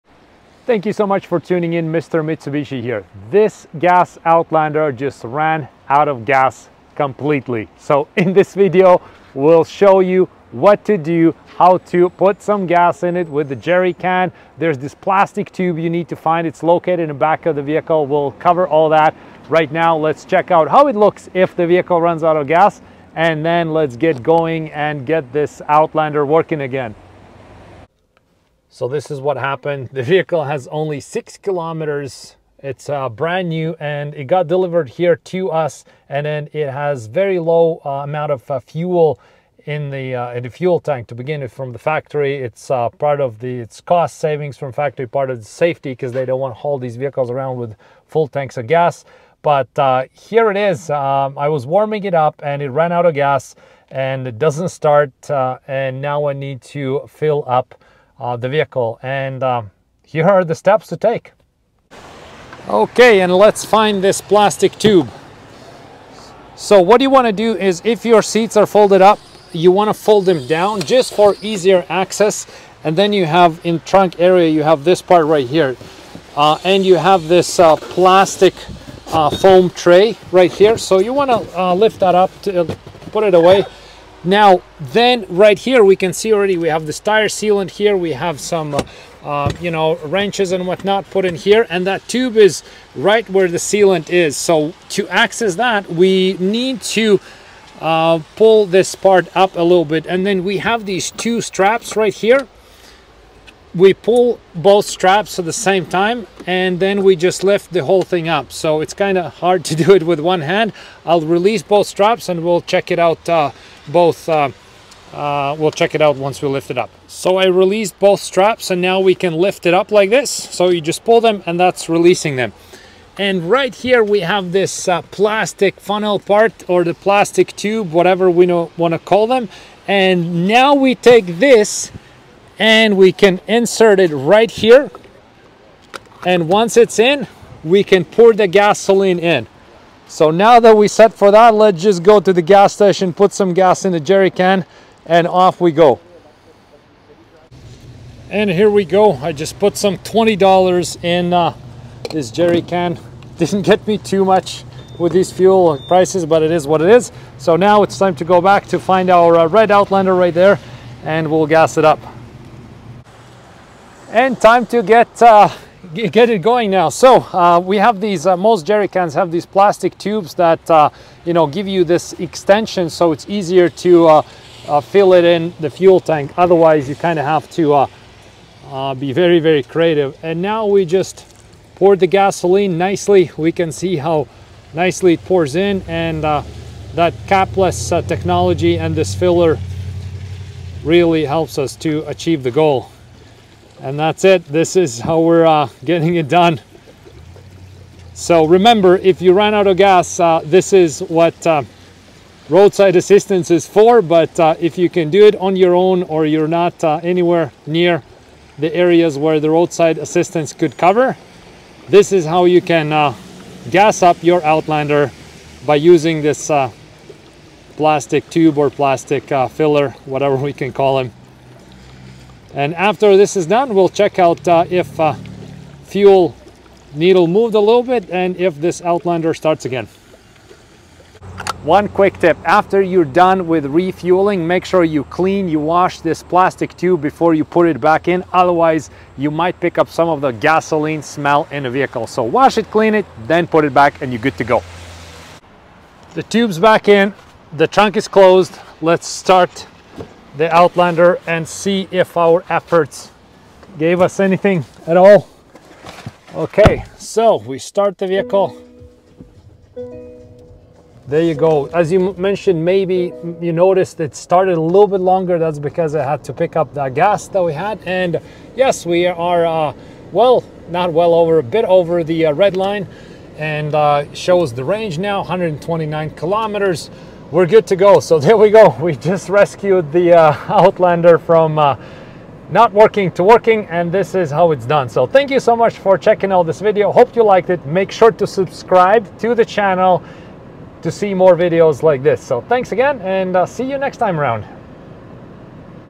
Out of gas..? Mitsubishi Outlander 2024
What to do if your Mitsubishi Outlander ran out of gas? How to fill it up? Outlander has capless fuel filler system system - where to find the plastic tube / fuel filler funnel so you can actually refuel it on the side of the road?